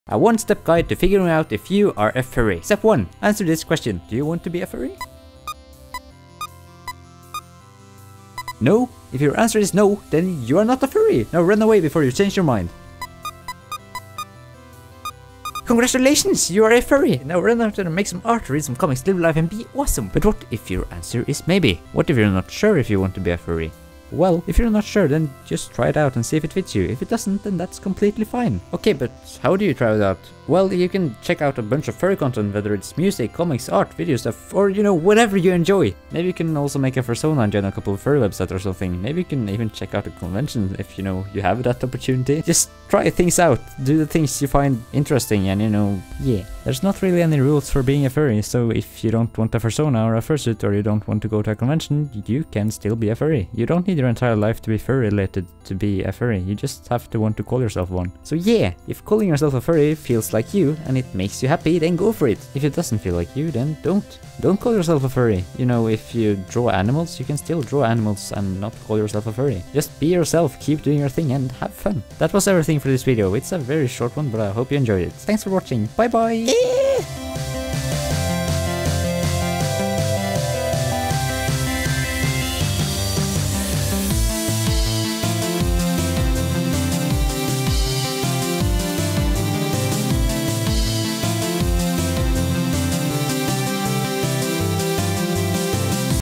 How to figure out if you are a Furry
Have you ever wondered about whether or not you are a furry? Are you a furry and just wants to be 100% sure that you are one? Are you browsing youtube and just want something to watch? Well then this video is just the right one for you.

Music made by Laffe the Fox! :D

Coffee drawing is made by Cinder! :D

If you read all this you deserve a fulfilling life with lots of awesome fuzz Σ(ノ°▽°)ノ